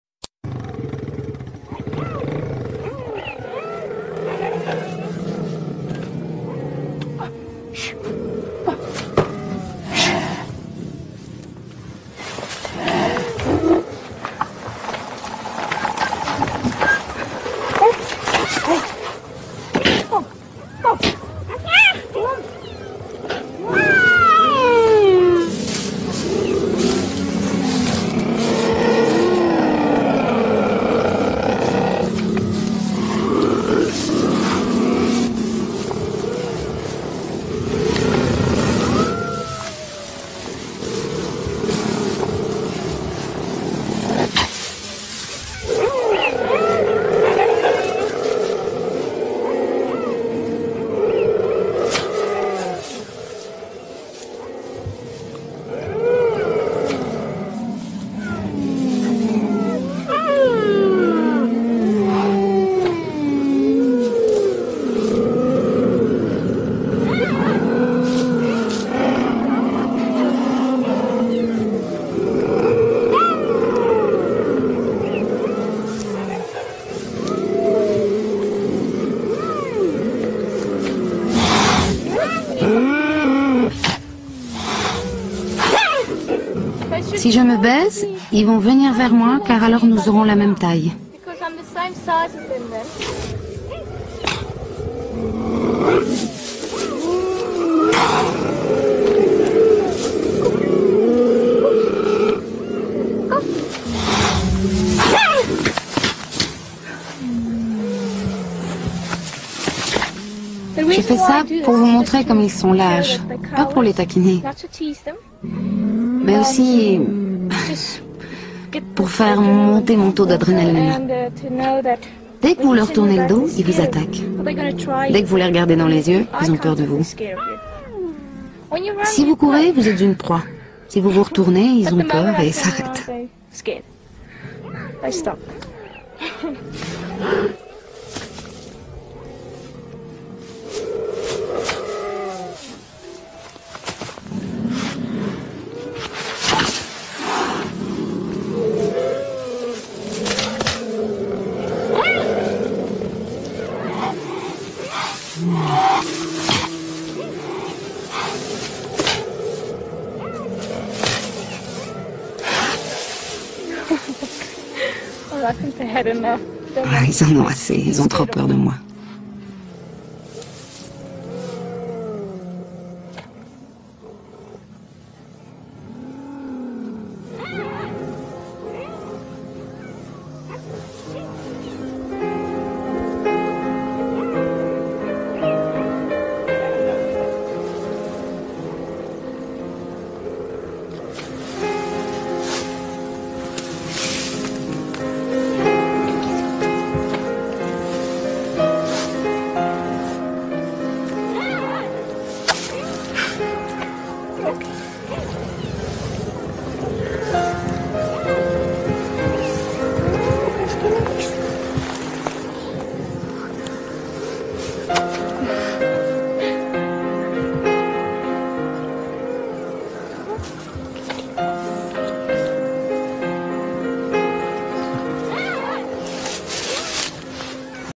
Girl stucked in National Park, How she handles Cheetahs..
This video was captured by a guy on his cell phone at Kruger National park, Mozambique.
The video was sold to Nat Geo for 1 million dollars.
Declared as one of the best short video on wildlife by animal lovers.
The girl in this video has some guts.
Quite an amazing stuff!